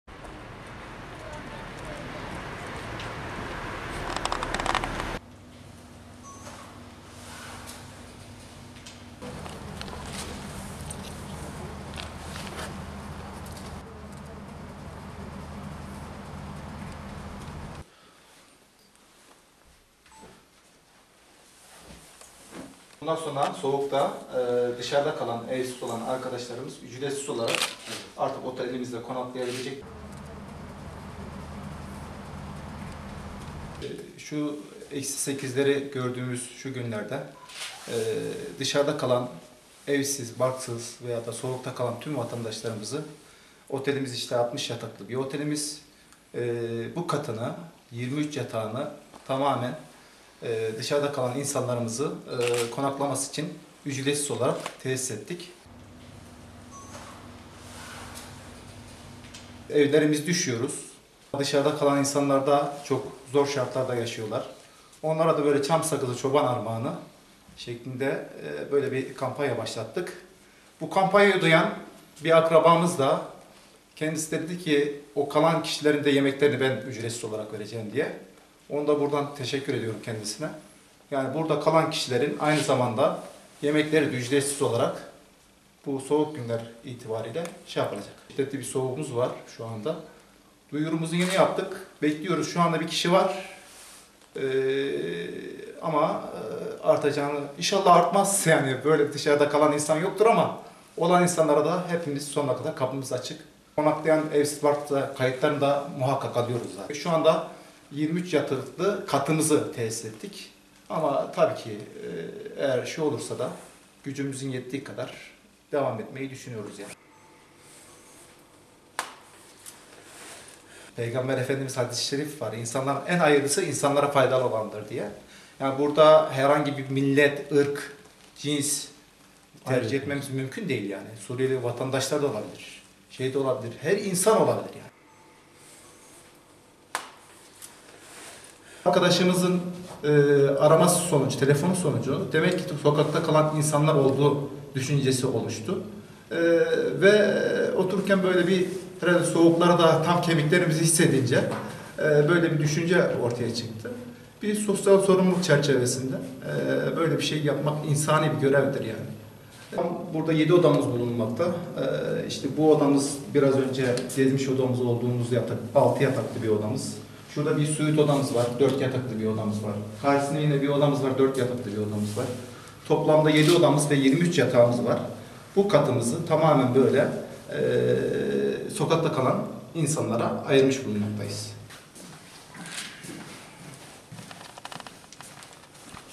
Otelini evsizlere açtı
Gaziantep’in İslahiye ilçesinde, iş insanı Mustafa Gelebek (41), otelinin bir bölümünü soğuk havada dışarıda kalan evsizlere açtı.

İlçede başlayan kar yağışı ve hava sıcaklığının eksi 8 dereceye düşmesi ile sokakta kalan evsizler, sığınacak yer aramaya başladı. İslahiyeli iş insanı Mustafa Gelebek ise işlettiği 60 yataklı otelin bir katındaki 7 odayı ve 23 yatağı, evsizlere açtı. Otelde, kış boyunca evsizlere kalacak yer, sıcak yemek imkanı sağlamak isteyen Gelebek, otelin girişine ise ‘Soğuk geçen şu günlerde sokakta kalan tüm insanlarımız ücretsiz olarak, Gelebek otelde konaklayabilecekler' yazısı astı. İslahiye Belediyesi, Kaymakamlık ve diğer kurum ve kuruluşlarında kendilerine böyle bir başvuru yapıldığında otele yönlendireceklerini ve zabıta ekiplerinin dışarıda kalan evsizleri otele getireceğini söyleyen Gelebek, "Peygamber efendimizin hadisi Şerifi, 'insanların en hayırlısı, insanlara faydalı olanıdır' inancıyla kış boyunca dışarıda kalmak zorunda kalan evsizleri otelimizde ağırlayacağız" dedi.

OTELDE 23 YATAK EVSİZLERE AYRILDI

Arkadaşından gelen telefonla evsizlere otelinin kapılarını açtığını ifade eden Gelebek, "7 yıldır otel işletmeciliği yapıyorum. İlçemizde eksi 8’leri gördüğümüz bu günlerde, soğukta, dışarıda kalan evsizlere, vatandaşlarımıza otelimizin kapılarını açtık. Otelimizin bir katında bulunan 7 oda ve 23 yataklı kısmını evsizlerimizin konaklaması için ücretsiz olarak ayırdık. Biz evlerimizde üşüyoruz, dışarıda kalan insanların çok zor şartlarda yaşadıkları malumunuz. Onlara da bu sert geçen kış günlerinde çam sakızı çoban armağanı misali böyle bir kampanya başlattık. Otelimizde konaklayacak tüm evsizlere sıcak yemek ikramı ile sıcak su ihtiyaçlarını karşılayacağız. Şu anda bir kişi var. Arkadaşım beni arayarak 'evsiz dışarıda kalan bir vatandaşımız var onu otelinizde ağırlayın, ücretini ben ödeyeceğim' dedi. Bunun üzerine dışarıda kalan evsiz insanlarımız var düşüncesi oluşunca, biz de soğukları kemiklerimize kadar hissettiğimiz bu günlerde böylesine bir sosyal sorumluluk çerçevesinde projeye imza attık" şeklinde konuştu.